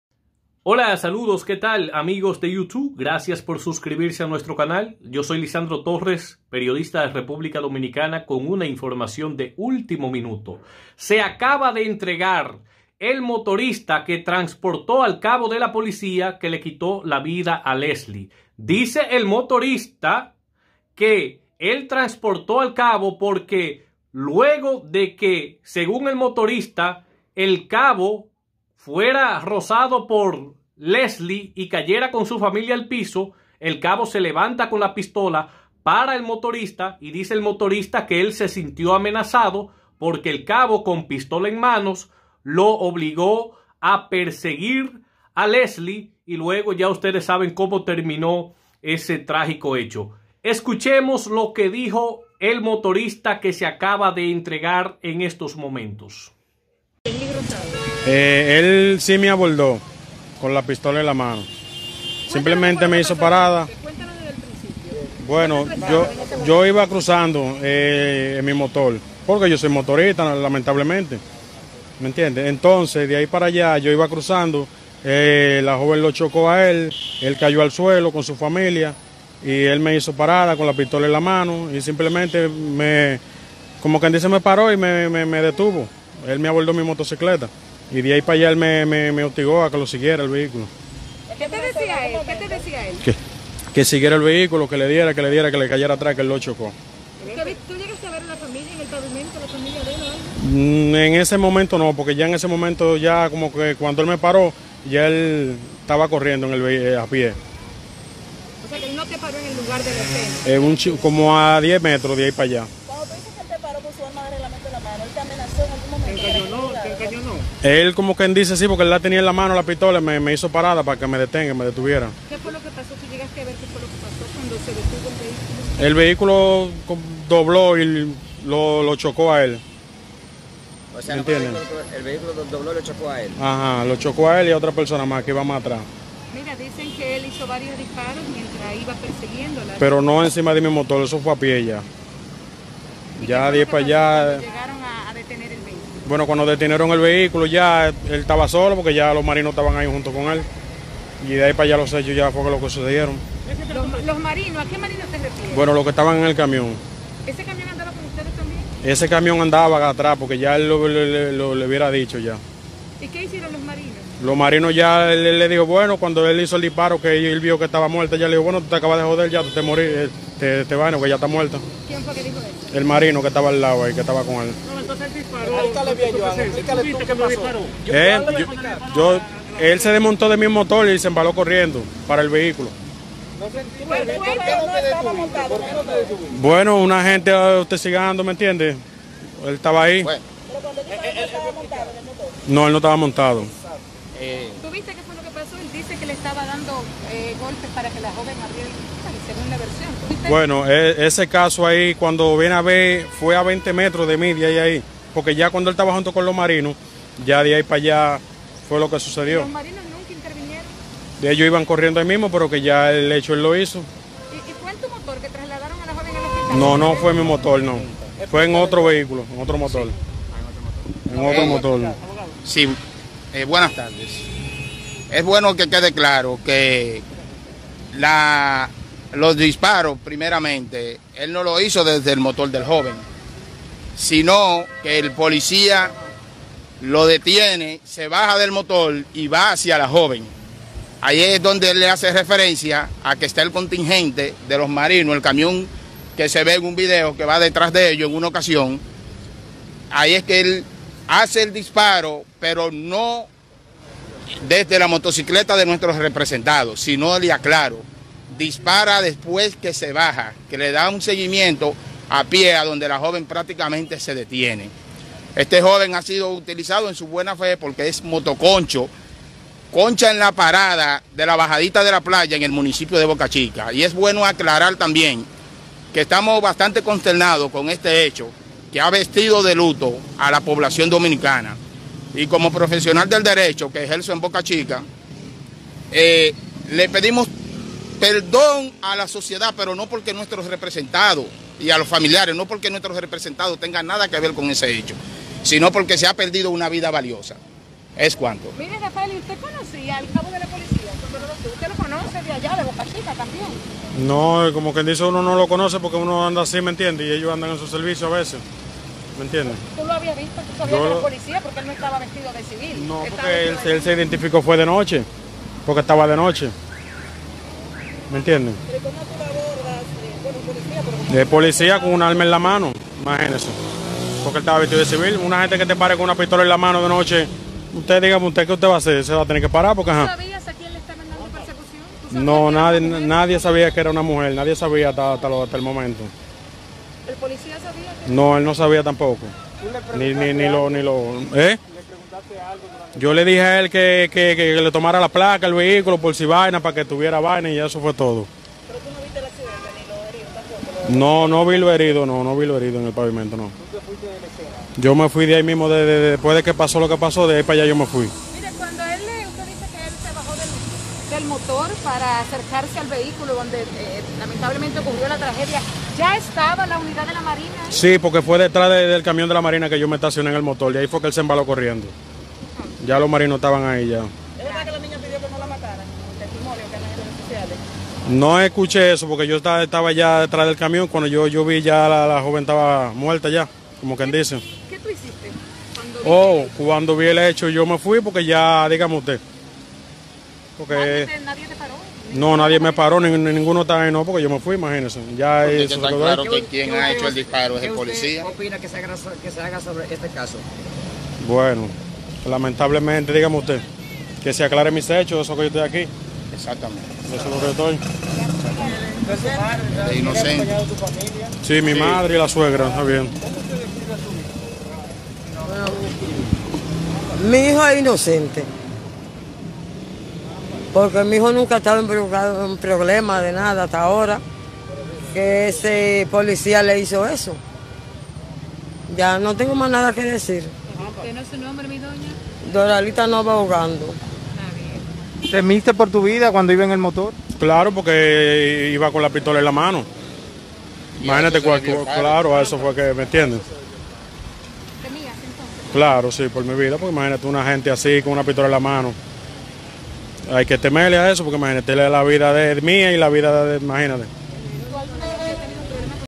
ÚltimoMinuto: Se entrega motorista que transportó al cabo en caso Leslie
Se entregó la tarde este martes el motorista que transportó al cabo de la Policía Nacional en el caso